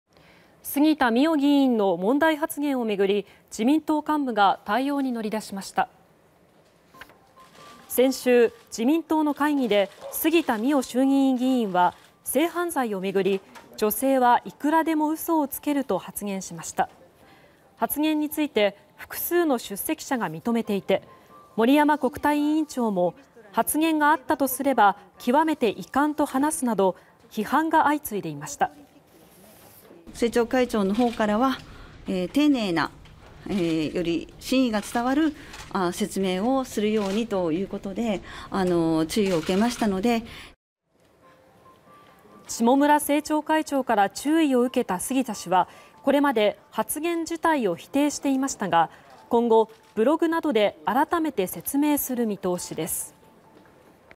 「女性は嘘つける」発言の杉田議員に自民幹部が注意(2020年9月30日)
杉田水脈衆議院議員の発言問題を巡り、自民党幹部が対応に乗り出しました。

　先週、自民党の会議で杉田議員は性犯罪を巡って「女性はいくらでも嘘をつける」と発言しました。発言について複数の出席者が認めていて、森山国対委員長も「発言があったとすれば極めて遺憾」と話すなど批判が相次いでいました。
　自民党・杉田水脈衆院議員：「政調会長の方からは、丁寧な、より真意が伝わる説明をするように注意を受けた」
　下村政調会長から注意を受けた杉田議員はこれまで発言自体を否定していましたが、今後、ブログなどで改めて説明する見通しです。